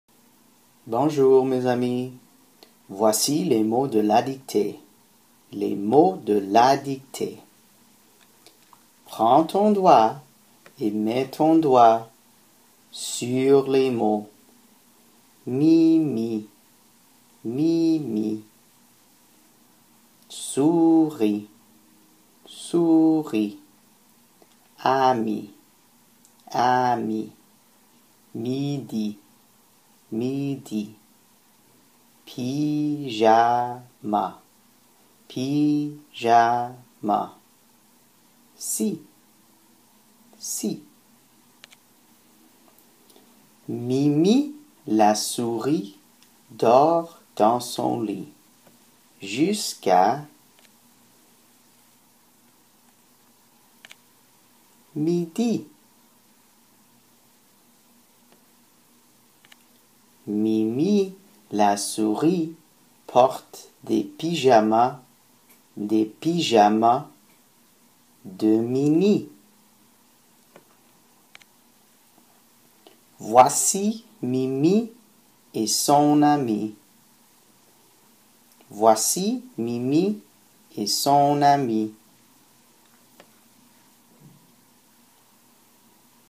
Le son “i” ou “y"
Les mots de la dictée.

Mimi la souris dort dans son lit jusqu’à midi. Mimi la souris porte des pyjamas de Minnie. Voici Mimi et son amie*.

*ami(e) can be masculin or feminin.